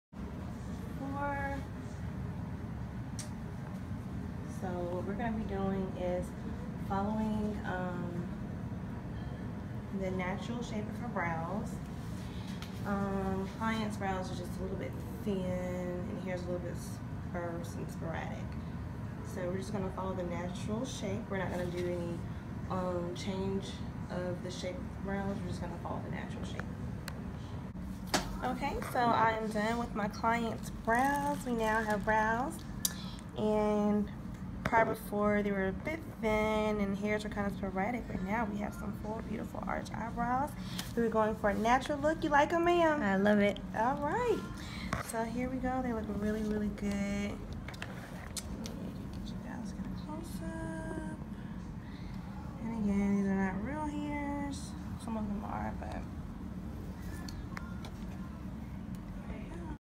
Client loss eyebrow hairs as a result of Alopecia ! Microblading was an awesome choice!
Microblading Thin &  Sporadic eyebrows to  naturally arched eyebrows.